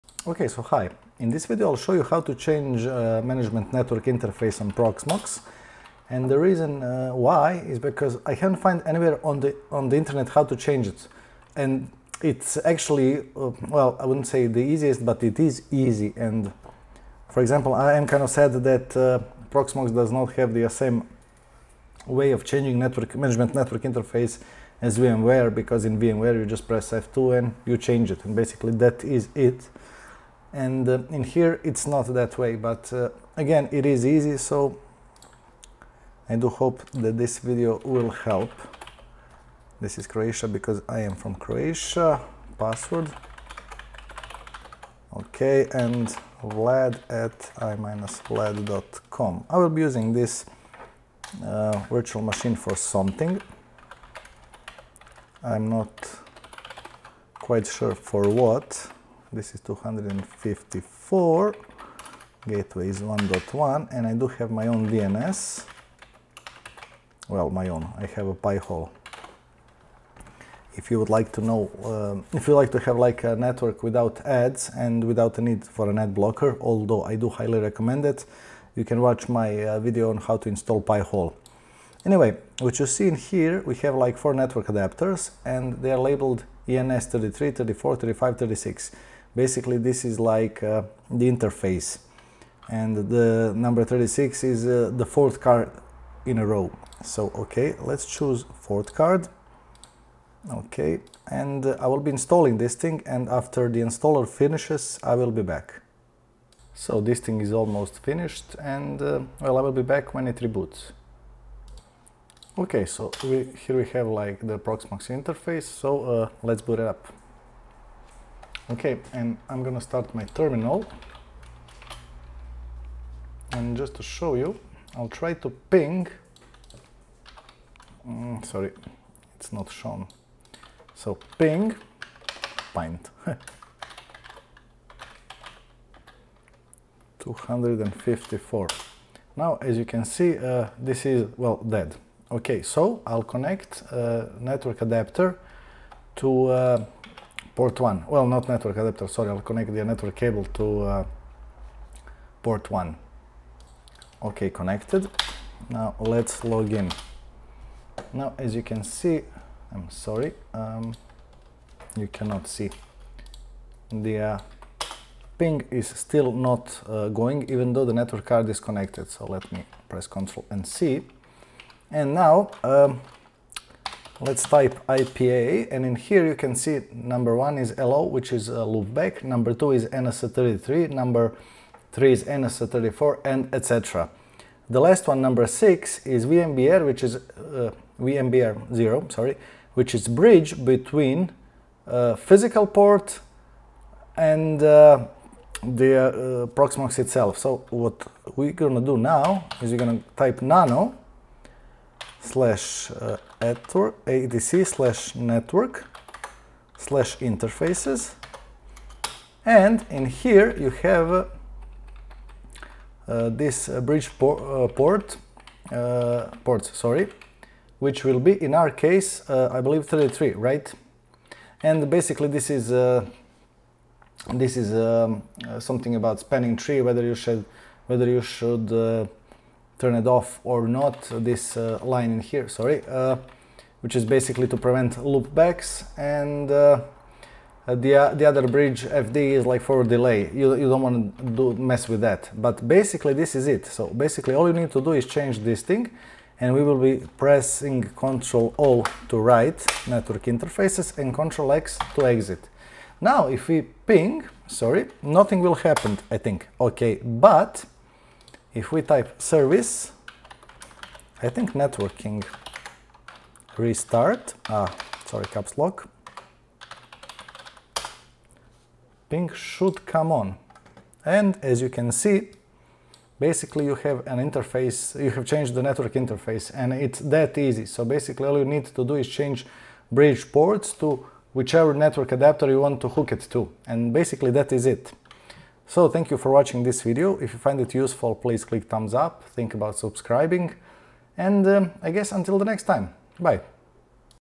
How to change network management interface in ProxMox
As title says: How to change network management interface in ProxMox

1. Go to your proxmox interface (either physically or through ILO)
2. Login
3. Type nano /etc/networking/interfaces
4. Change the line bridge -ports ens36 (in your case it can be eno1 or some other name) and change it do desired adapter

- If you don't know the label of interface before editing
/etc/networking/interfaces file type: ip a

your interface name will be by the ordinal number so, for example

1. lo Loopback
2. eno1
3. eno2

...etc

If you have any questions ask them down below!

BTC-ME:
1P7drxDQALyJDm8rCVSuhEXkiS5LnYFpRr

ETH-ME
0x21E4cC6BEf890000B1cD52e23F426382d5651B92